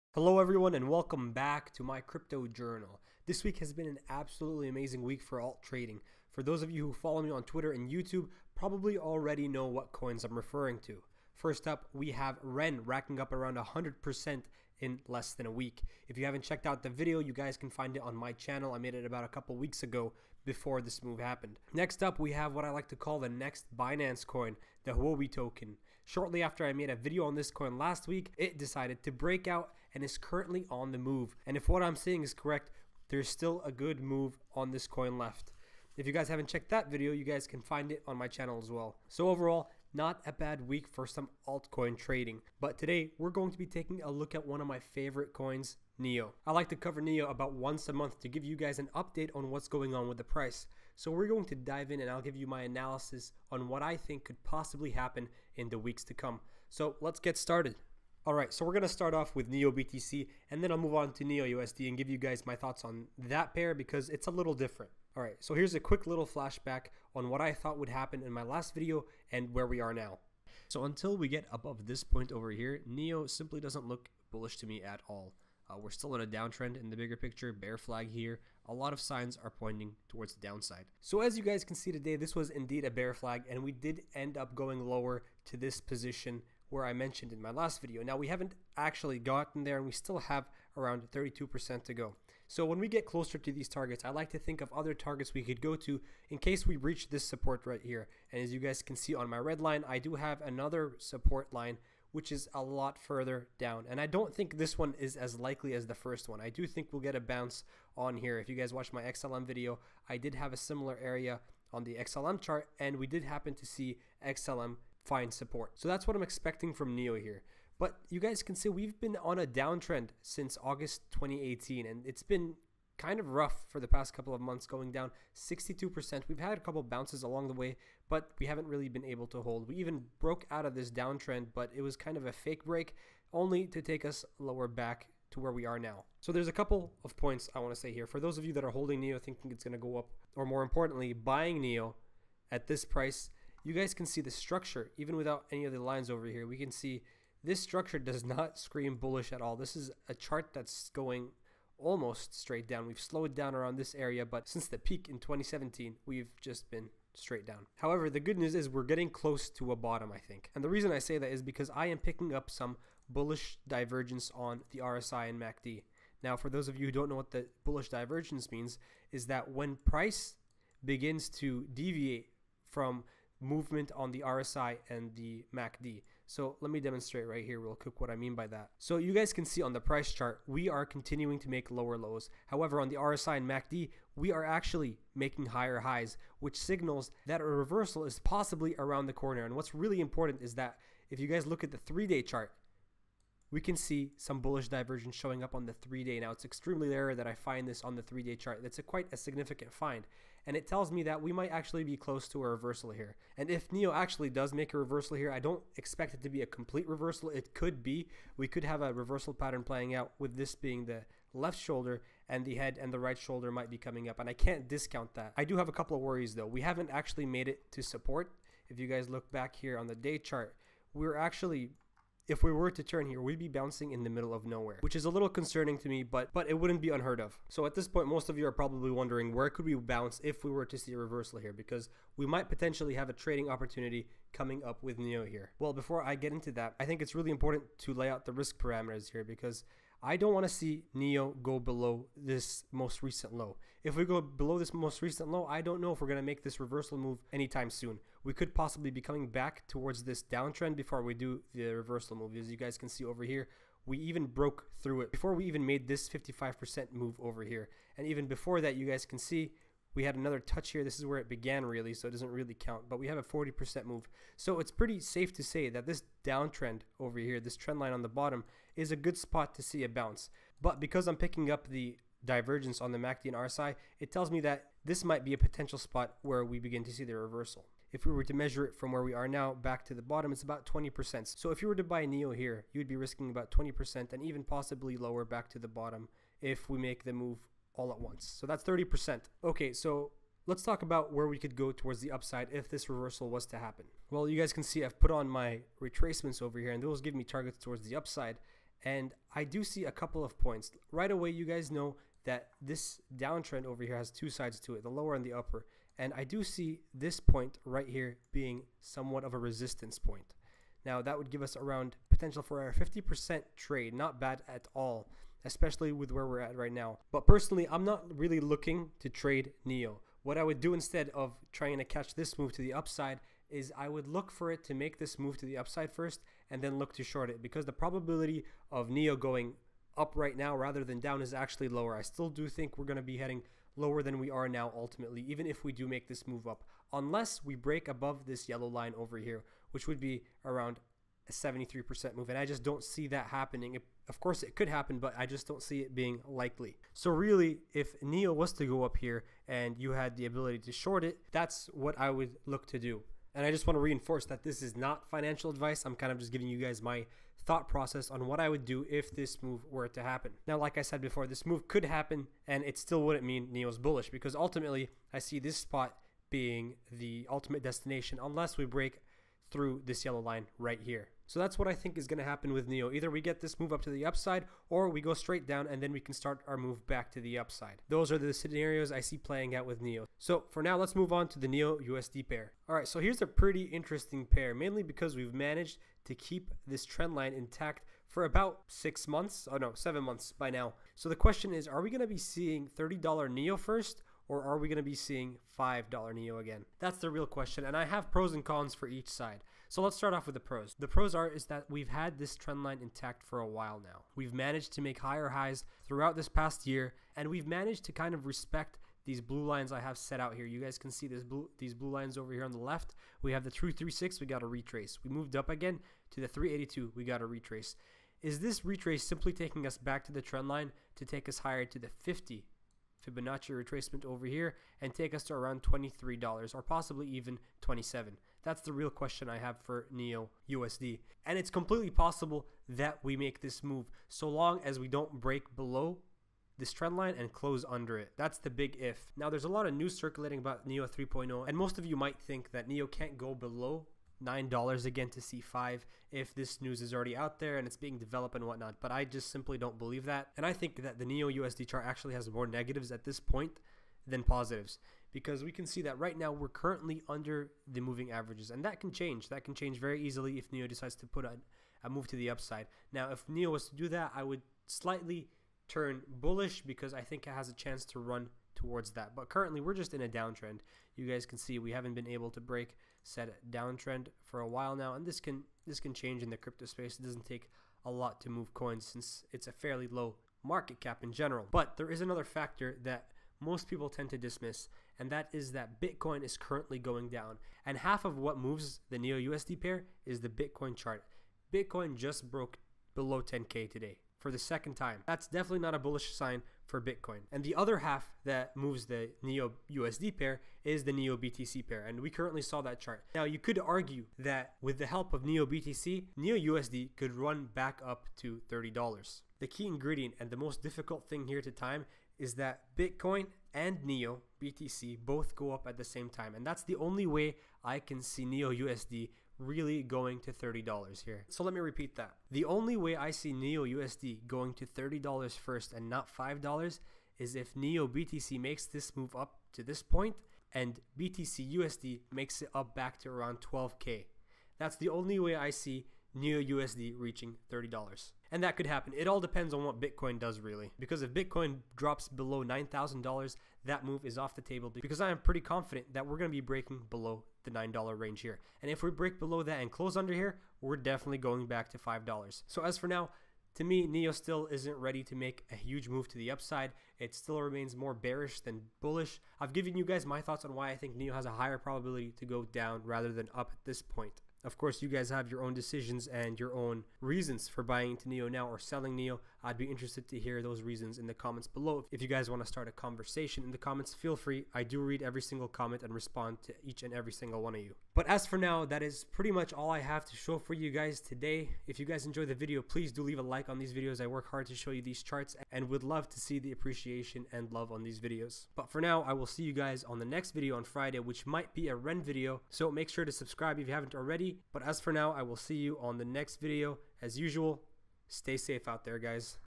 NEO 3.0 - What Comes First $5 or $30? (2019)
This week has been quite amazing when it comes to the alts if you picked the right ones. If you've been following my channel/Twitter you would know that I've had my eyes on REN & Huobi. Since making my videos both these coins have made significant gains.

In todays video I'll be taking a look at one of my favorite coins, NEO. NEO has had a rough time in this bear market and I've covered NEO in other videos in the past. Not much has changed, but I do like to give you guys an update on NEO every month or so with updated thoughts and analysis.

NEO/BTC is showing some interesting signs of possibly giving us a bounce to the upside. NEO/USD has an interest chart structure however ultimately I don't know if we'll be seeing $30. I lay out all my reasoning in the video.

Are you currently holding NEO? If so what price did you buy at?